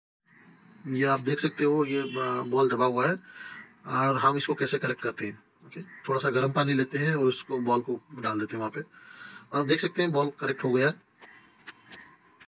How to Fix a Dented/pit/ squashed/crushed Ping Pong Ball
1. Fill a bowl with warm water and add a few drops of dish soap.

2. Place the dented ping pong ball in the bowl and let it soak for 10 minutes.

3. Remove the ball from the bowl and gently rub the dent with your fingers.

4. Place the ball on a flat surface and press down on the dent with your thumb.

5. Roll the ball around on the flat surface to help the dent pop out.

6. Rinse the ball off with cold water and let it air dry.

how to
ping pong tricks
ping pong trick shots
table tennis table
# tennis tricks.